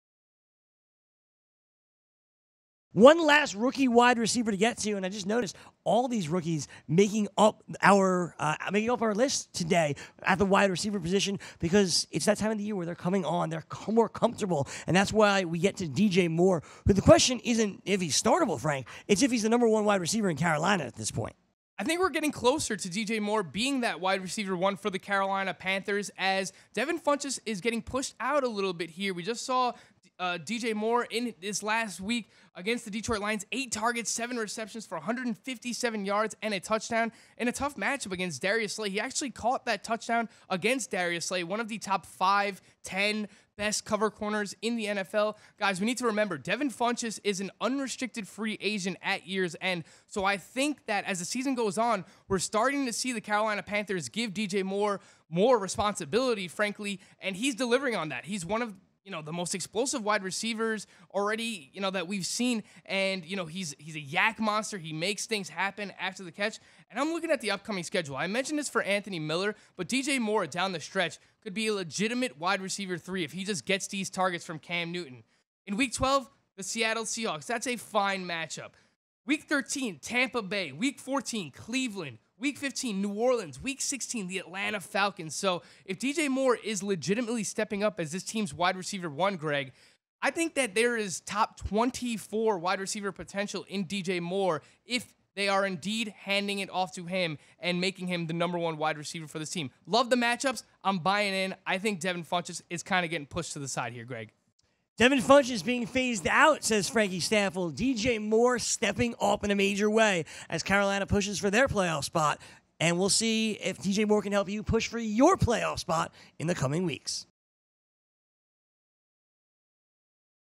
FanDuel Hurry Up: DJ Moore
Has D.J. Moore passed Devin Funchess as the number 1 WR in Carolina?

Gregg and Frank take a look on FanDuel Hurry Up.